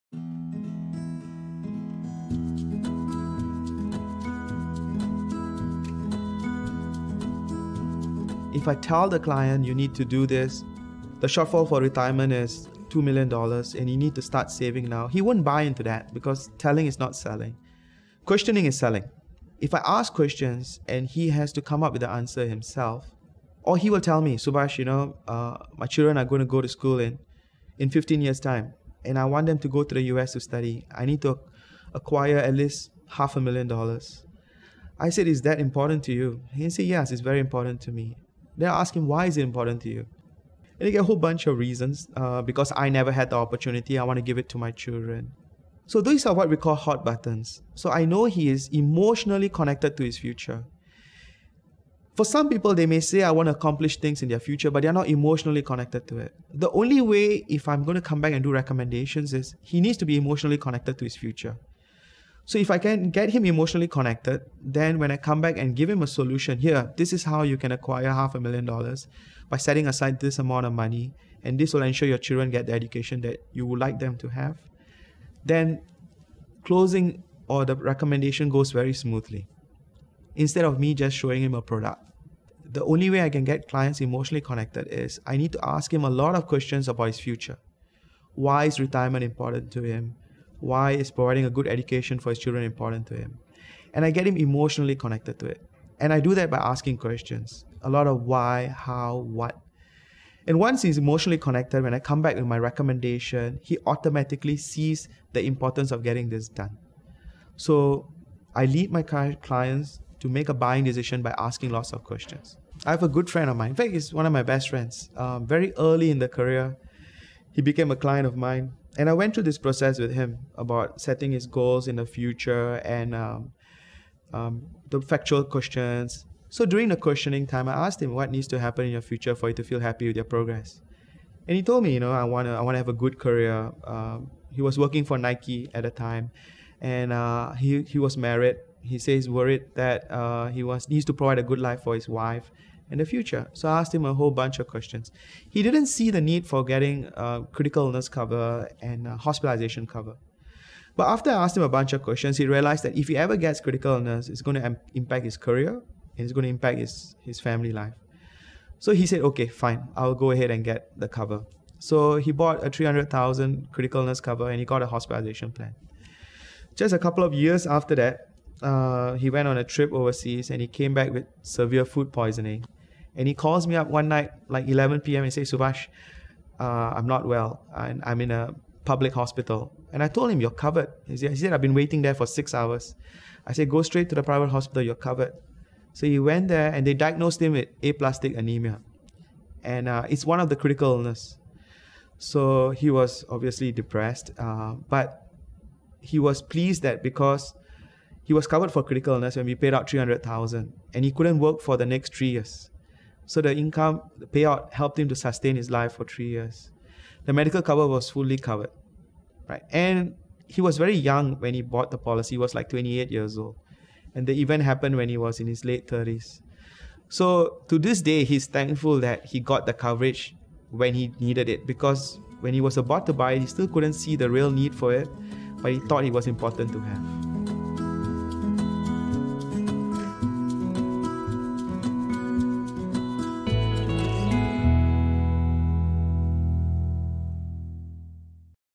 Telling is not selling
Ask your clients questions so they come up with the answers about what’s important to them. Then you can guide clients in their financial planning. Find out more from that MDRT member Subhas V. Nathan, of Singapore.

You’ll find more ideas from MDRT members that will change your business